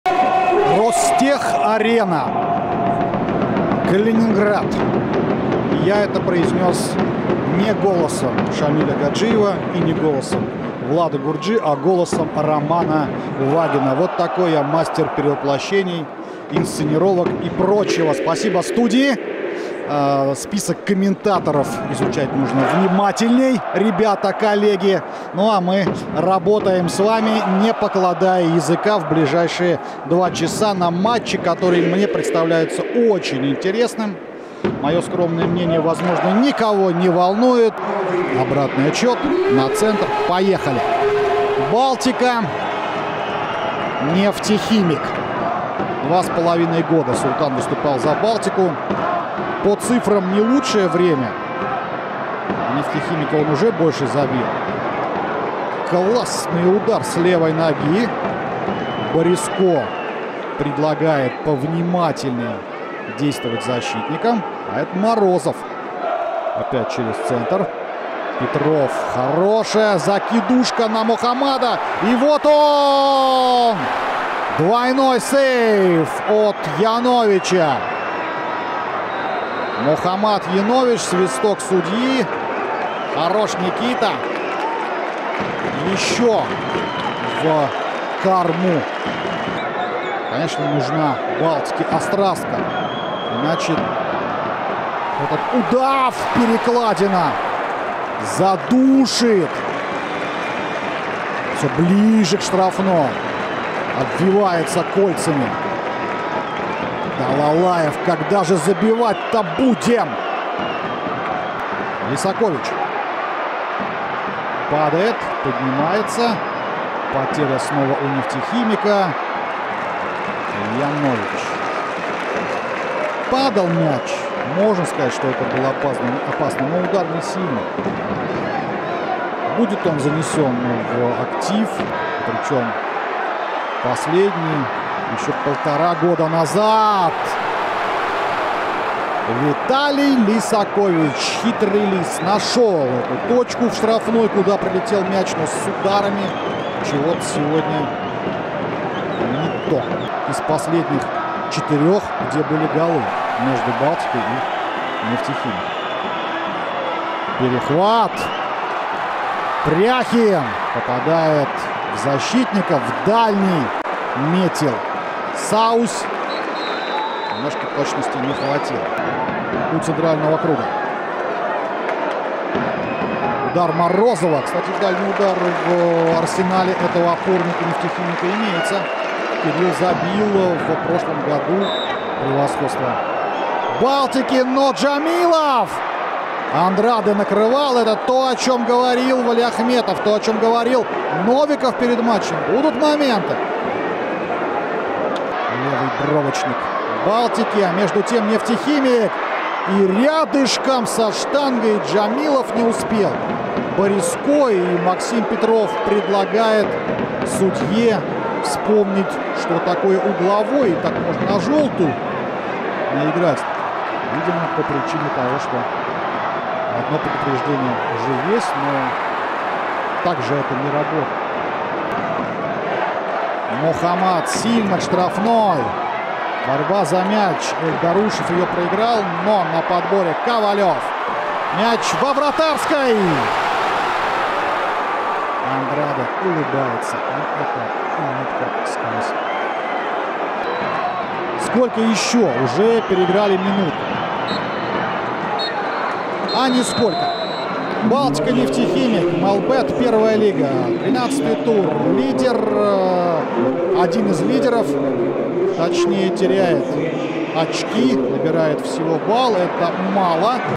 Обзор матча «Балтика» — «Нефтехимик» | 13 тур МЕЛБЕТ-Первой Лиги
Лучшие моменты из игры «Балтика» (Калининград) и «Нефтехимик» (Нижнекамск) 6 октября 2024.